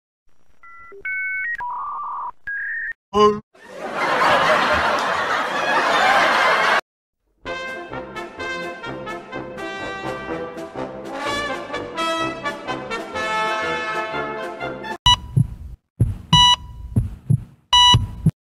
man gets penis stuck in car door
the title is accurate